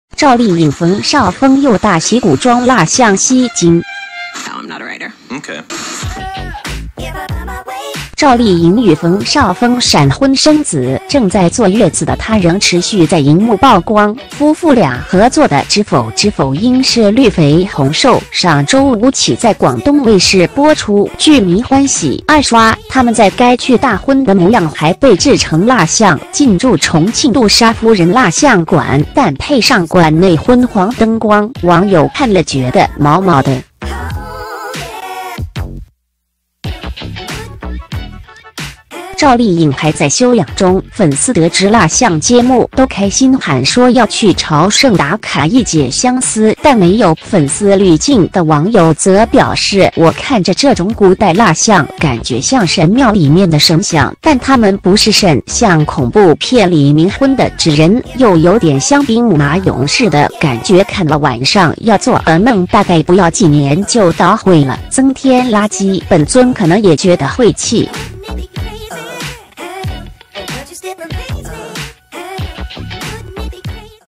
趙麗穎馮紹峰又大喜 古裝蠟像吸「驚」
趙麗穎與馮紹峰閃婚生子，正在坐月子的她仍持續在螢幕曝光，夫婦倆合作的《知否知否應是綠肥紅瘦》上周五起在廣東衛視播出，劇迷歡喜「二刷」，他們在該劇大婚的模樣還被製成蠟像，進駐重慶杜莎夫人蠟像館，但配上館內昏黃燈光，網友看了覺得「毛毛的」。
趙麗穎還在休養中，粉絲得知蠟像揭幕，都開心喊說要去朝聖打卡一解相思，但沒有「粉絲濾鏡」的網友則表示，「我看著這種古代蠟像，感覺像神廟裡面的神像，但他們不是神」；「像恐怖片裡冥婚的紙人，又有點像兵馬俑似的，感覺看了晚上要作惡夢」；「大概不要幾年就搗毀了，增添垃圾，本尊可能也覺得晦氣」。